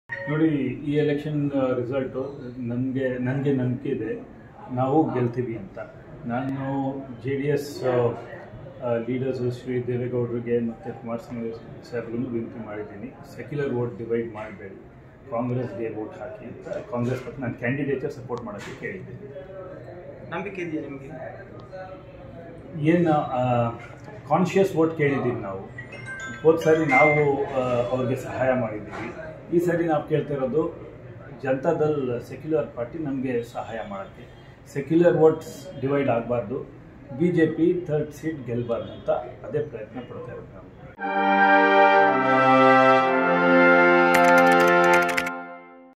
ರಾಜ್ಯಸಭಾ ಚುನಾವಣೆ | ಜೆಡಿಎಸ್ ನಾಯಕರಲ್ಲಿ ಆತ್ಮಸಾಕ್ಷಿ ಮತ ಕೇಳಿದ್ದೇವೆ, ಗೆಲ್ಲುವ ವಿಶ್ವಾಸ ಇದೆ: ಮನ್ಸೂರ್ ಅಲಿ ಖಾನ್
ಜೆಡಿಎಸ್ ನಾಯಕರಲ್ಲಿ ಆತ್ಮಸಾಕ್ಷಿ ಮತ ಕೇಳಿದ್ದೇವೆ, ಗೆಲ್ಲುವ ವಿಶ್ವಾಸ ಇದೆ : ಮನ್ಸೂರ್ ಅಲಿ ಖಾನ್

ರಾಜ್ಯಸಭಾ ಚುನಾವಣೆ